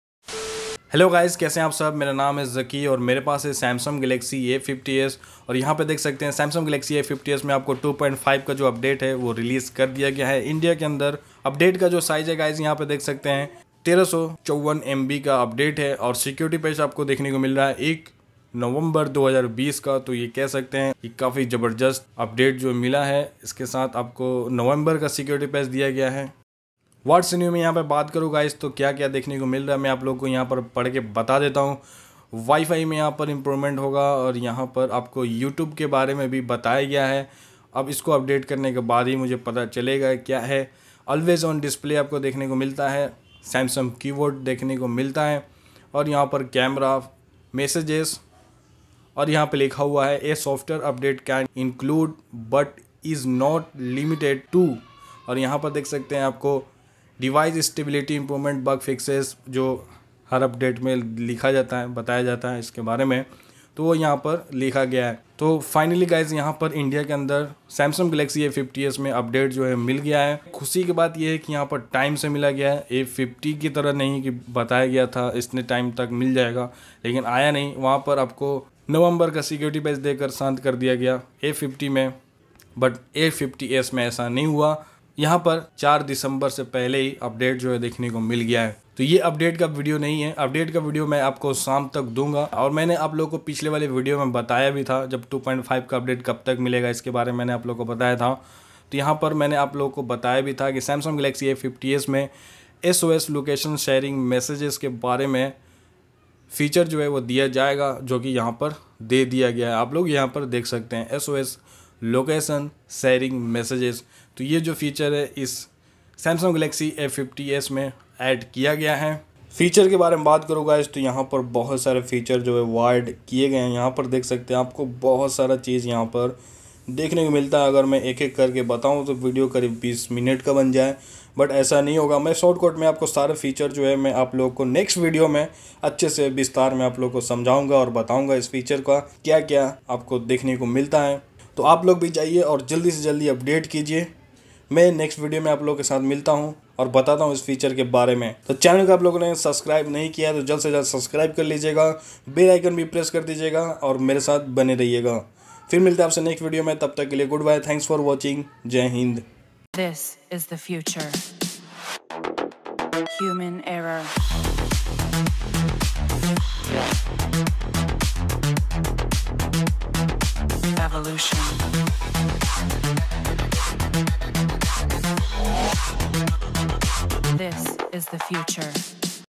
Samsung Galaxy A50s Got the OneUI 2.5 Update In India | November Security patch
hello guys samung galaxy a50s ko india me one ui 2.5 ka update mil chuka hain update ka size 1354mb ka hain or bahut sare new features dekhne ko milta hain. update ka video jaldi dekhne ko milega apko. channel ko subscribe kar k bell icon press kar dijiyega. video ko like share comments jarur se kar dijiyega,